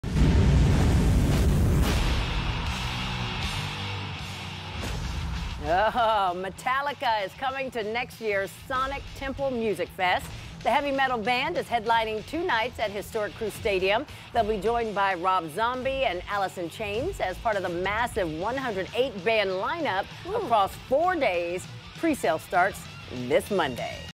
Metallica announced as headliner for 2025 Sonic Temple music festival
Organizers say the band was the most requested to act to play at the festival.